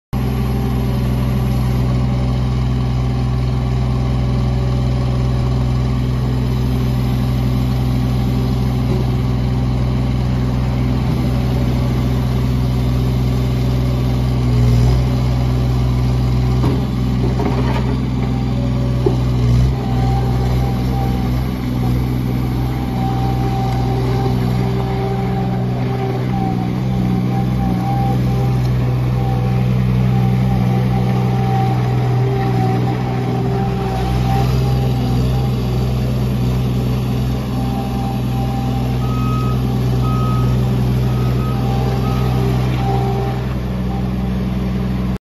2016 BOBCAT S70 For Sale
EROPS, 622 Hours, Heat, Hand & Foot Controls, AUX Hydraulics, 23.5 HP Kubota Diesel, 44" GP Bucket, Mini SSL Quick Attach, 7-12 Tires, 700 LBS Lift Capacity, 2795 LBS Operating Weight, Located at McGrew Equipment Company (Seven Valleys PA)

 [BOBCAT S70 SKID STEER LOADER]